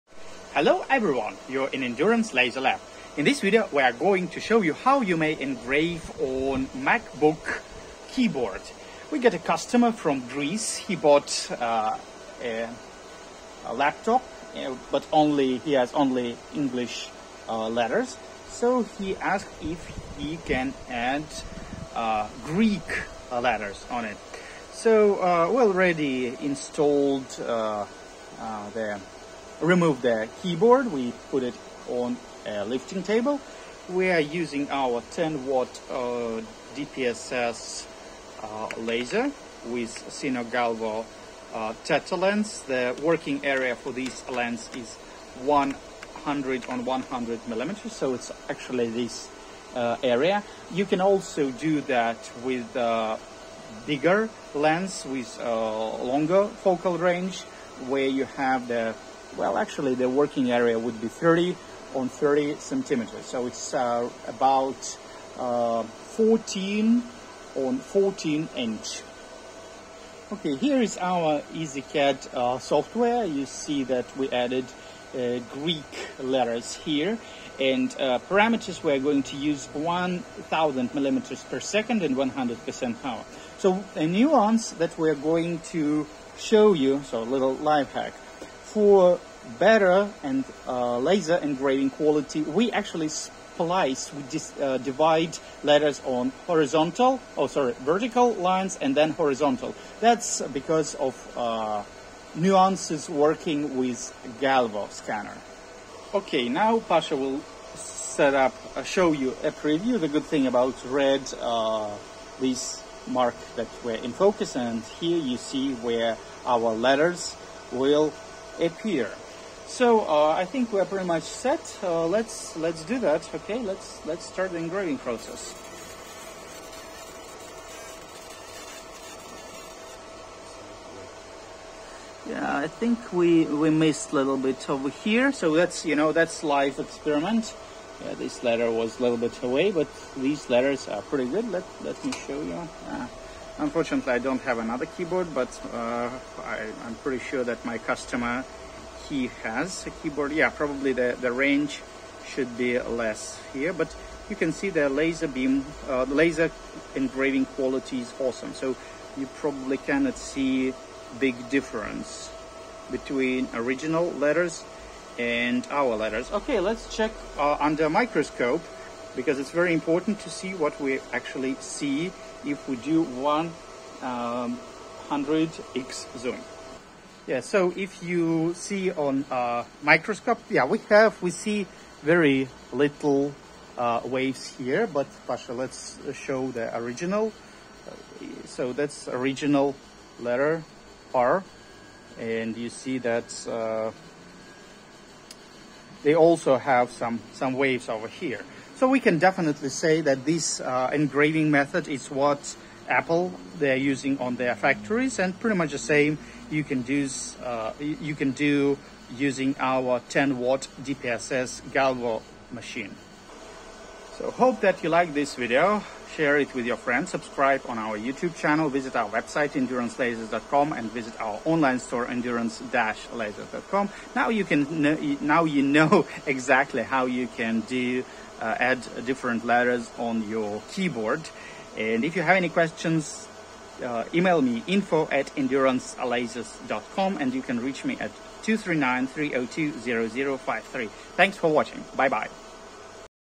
Adding letters on Apple Macbook keyboard with Endurance 10 watt DPSS laser
A detailed video how to add extra letter on Apply Macbook keyboard with a 10 watt Nd:YAG Endurance DPSSL with Galvo scanner.

Testing the final result (inspection with 100x digital microscope)

8285 Ibis Club Dr., Unit. 812